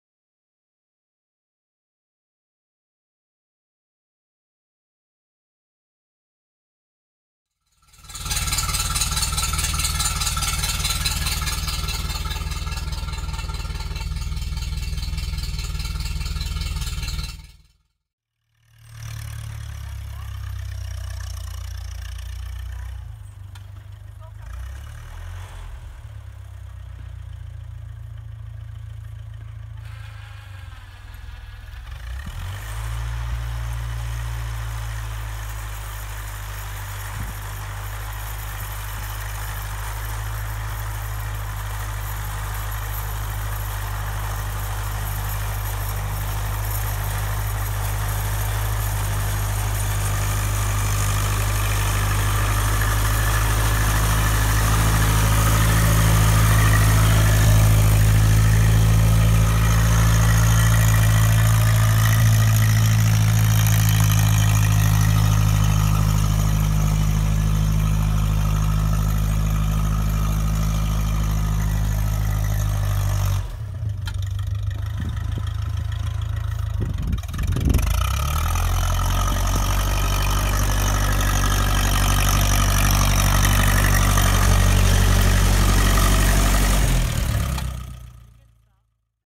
1955 Massey Ferguson Tea20 pushing snow
Here is my tractor in action on a cold winter day in northern Canada.